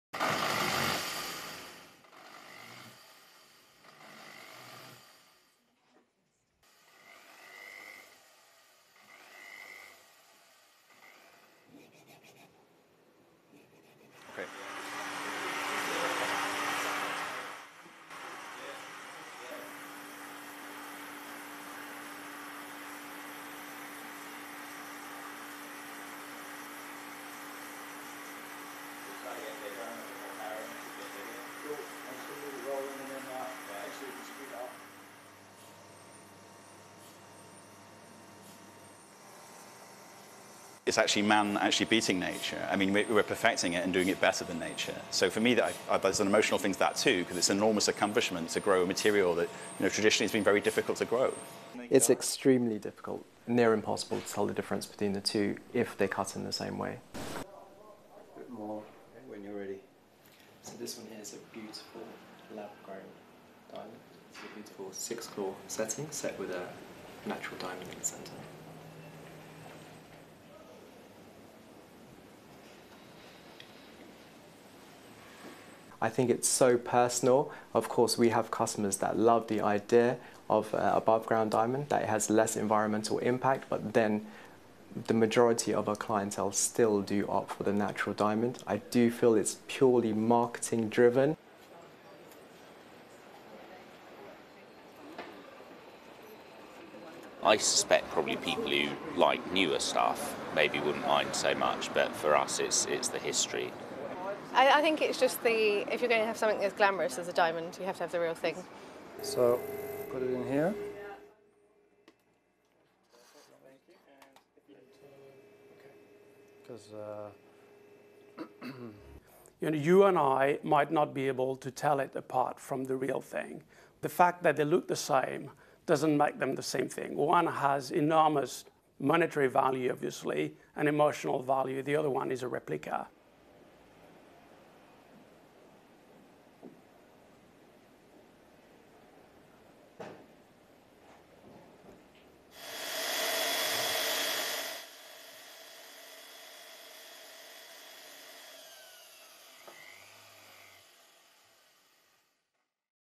Mystery of the diamond bearing meteorites is SOLVED Daily Mail Online
The mystery of the diamond-containing meteorites found across the world has finally been solved.

Scientists from RMIT and Monash University in Australia have discovered that the diamonds were formed in an ancient dwarf planet from our solar system.

The planet likely smashed into a giant asteroid about 4.5 billion years ago, which resulted in huge temperatures and moderate pressures.

ADVERTISEMENT
These conditions caused graphite in the space rock to undergo a process that turned it into lonsdaleite - a rare hexagonal form of diamond.

This was then partially replaced by regular diamond - a tetrahedral lattice of carbon atoms -  as the planet cooled and pressure reduced.

Professor Andy Tomkins, a geologist and lead author, said: 'Nature has thus provided us with a process to try and replicate in industry.

'We think that lonsdaleite could be used to make tiny, ultra-hard machine parts if we can develop an industrial process that promotes replacement of pre-shaped graphite parts by lonsdaleite.'